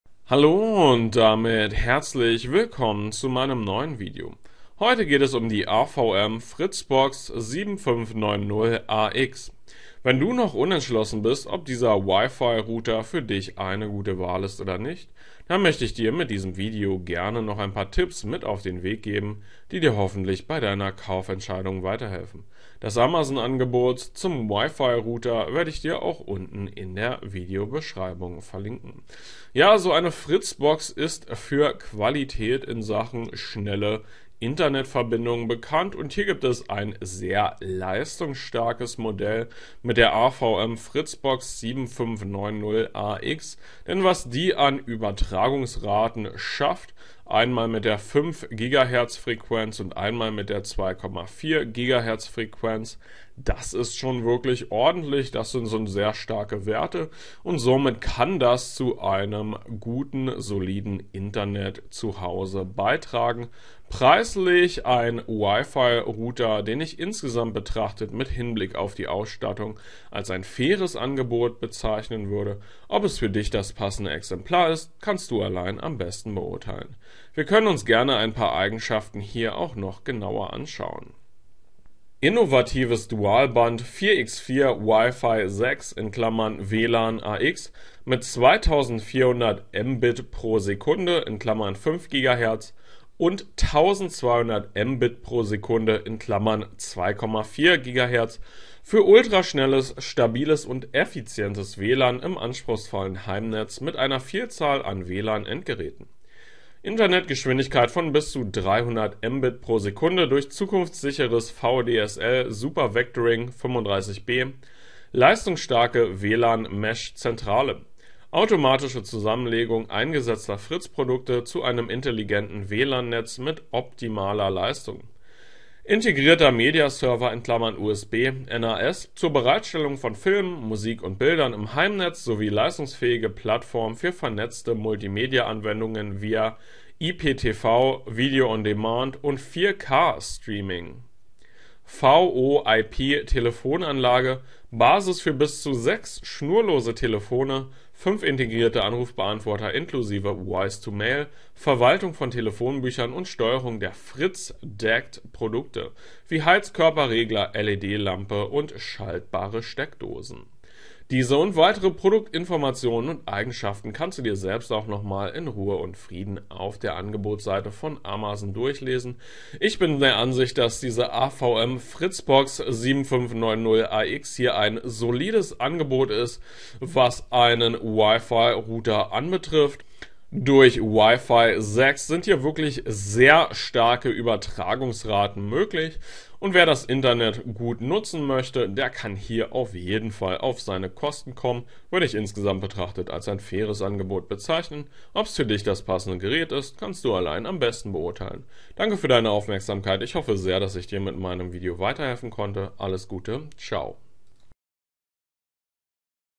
🐉 AVM FRITZ!Box 7590 AX Wi-Fi 6 Router: eine gute Wahl? 🐉
▶ Jetzt auf Amazon ansehen ▶

AVM FRITZ!Box 7590 AX (Wi-Fi 6 Router: Ist der Test eine gute Idee oder eher bleiben lassen? Wo kaufen? Was beachten? Hier gibts Tipps vom kleinen Drachen! :)